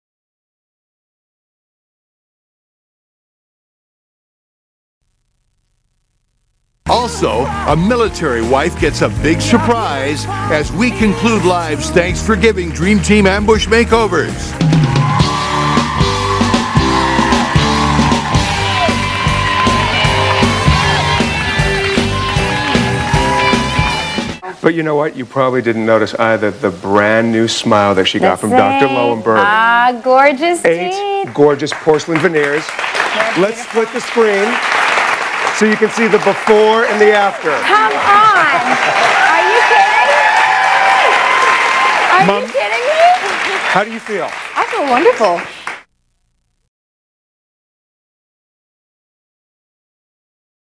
Smile makeover by New York Cosmetic Dentist for a bright white smile
Smile makeover by New York Cosmetic Dentist Dr. Marc Lowenberg for a bright white smile